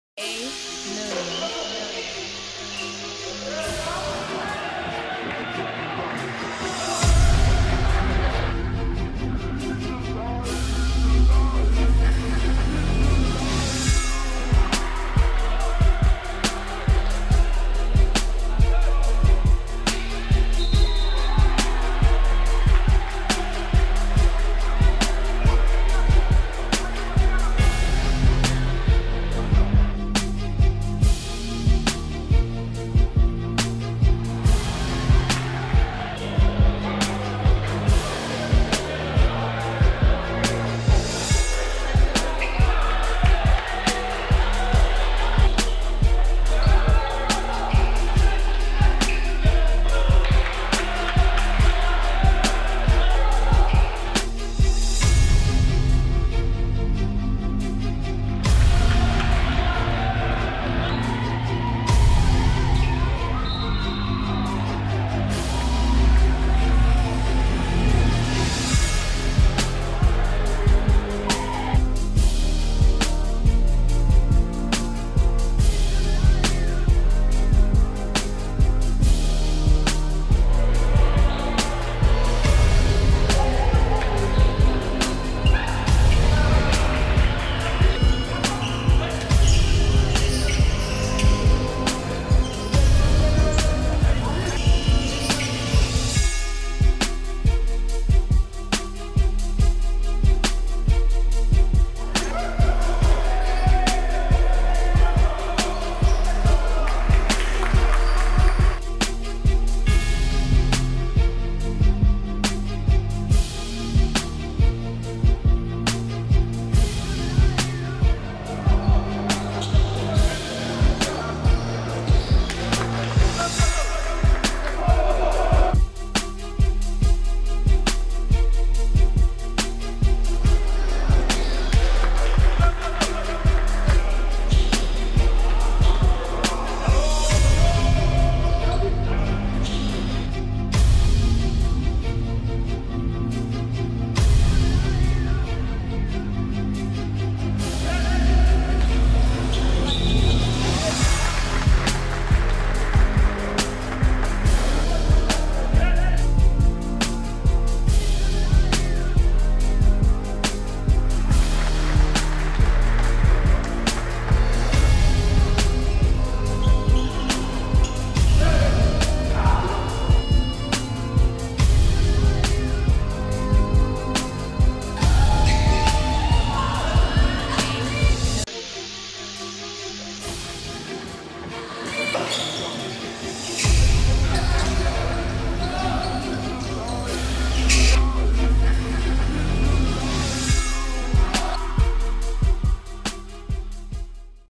BEST OF DAUD HOSEA IN RBA League 2015-2016
Watch how Daud Hosea help his team Chui in the second round of  RBA league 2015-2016